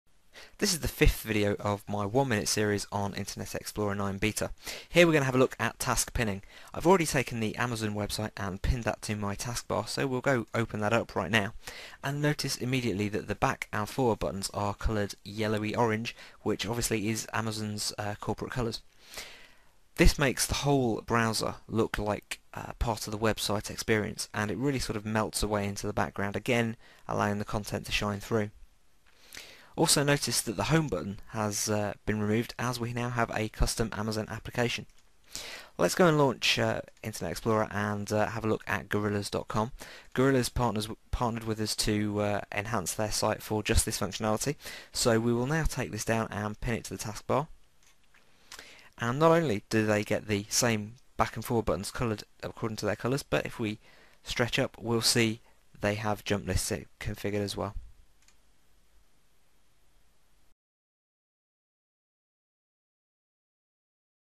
Internet Explorer 9 BETA - Video 5 - Tab pinning for Internet Explorer 9 BETA
A look at website pinning in the new Internet Explorer 9 Beta in just one minute